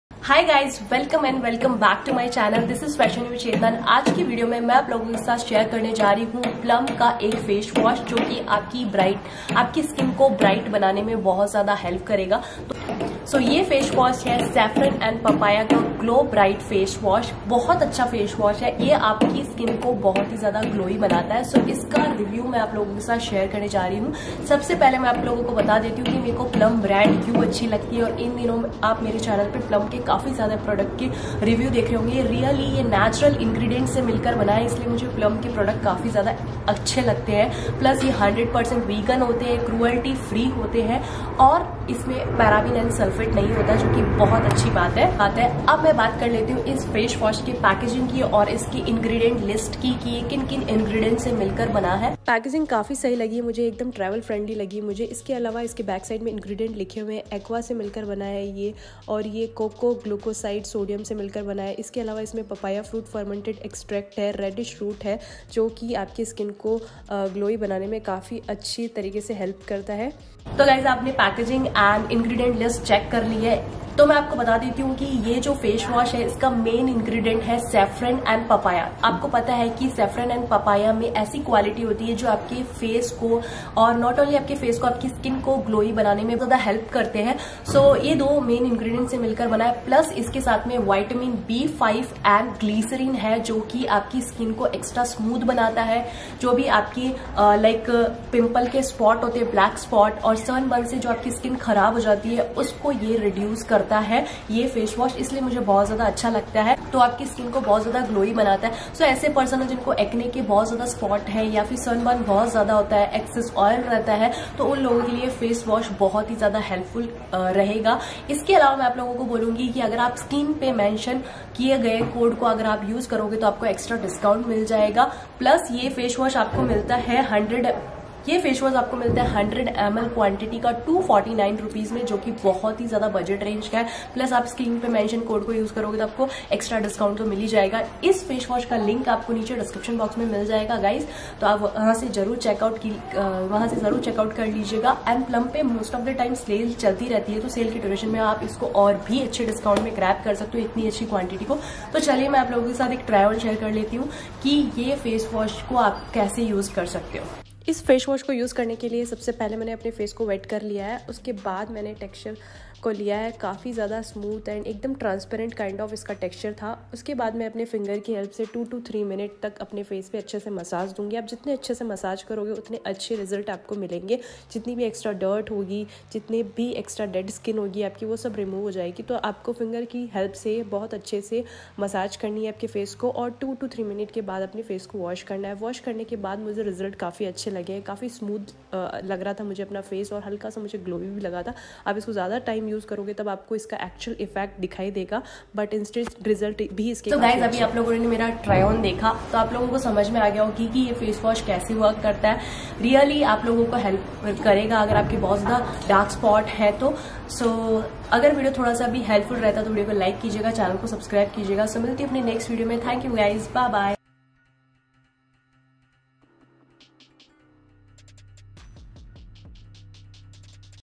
plum saffron & papaya glow bright face wash review | honest review for all skin type face wash
My Go-To Facewash For Glowing Skin | Say Goodbye to Dull & Tired Skin |

GlowBright is here to give you bright, glowing skin!
Plum's Saffron & Papaya Glow Facewash is the go- to face wash to fight dullness and get glowing bright skin!
With the goodness of Saffron, Papaya Enzyme, Vitamin B5 & Glycerin that enhances overall skin health & youthfulness by improving cell turnover. This gentle, non-drying sulphate-free face wash helps boost glow, fight dullness & dark spots to
make you glow like never before!
 This facewash is my skins, Dream Team. Check out the video for my detailed review on the face wash. From how to use it to what it does, I have covered it all.

avail 10% on all Plum products on their website!

Would love to know your favourite Plum products!